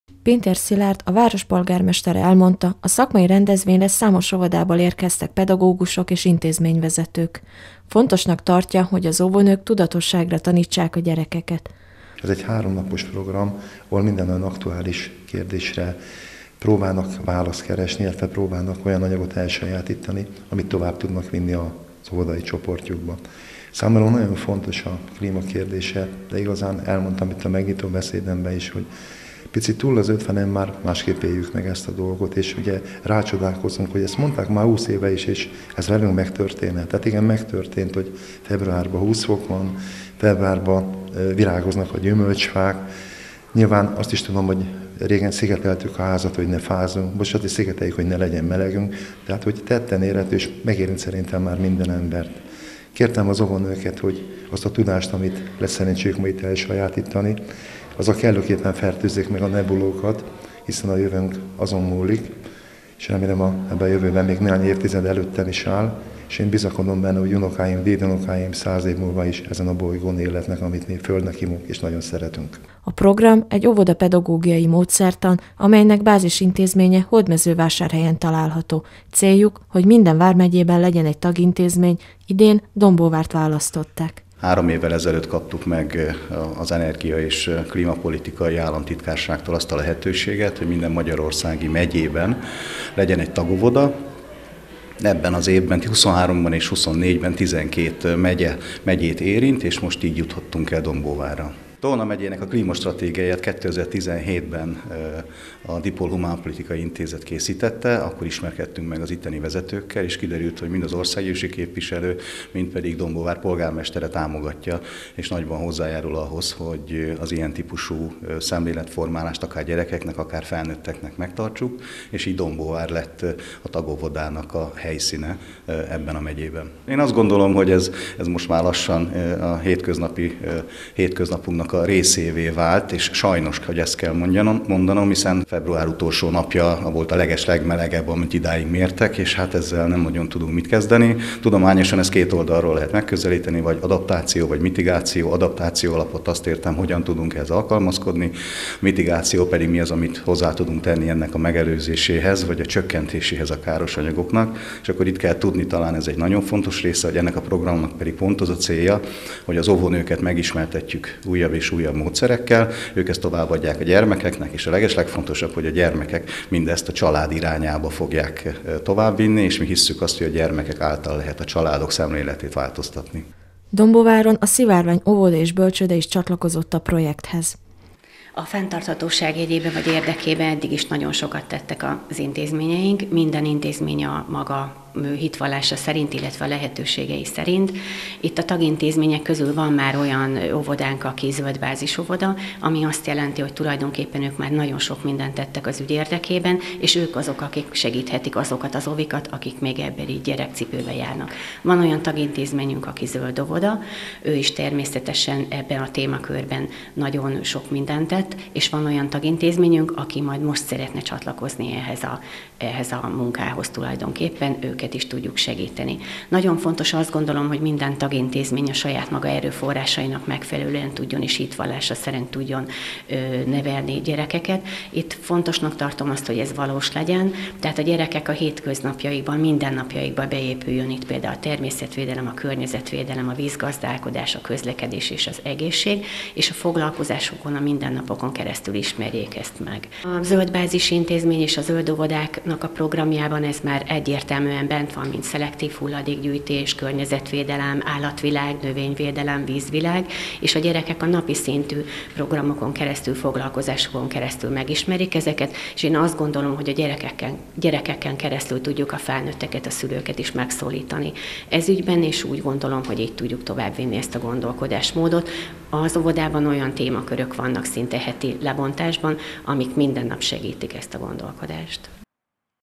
Klímanócskák - környezetvédelmi előadás Dombóváron
A Klímanócskák nevű programot, minden évben megrendezik országszerte a települések óvodái számára. Dombóváron az előadást a Német Házban tartották meg.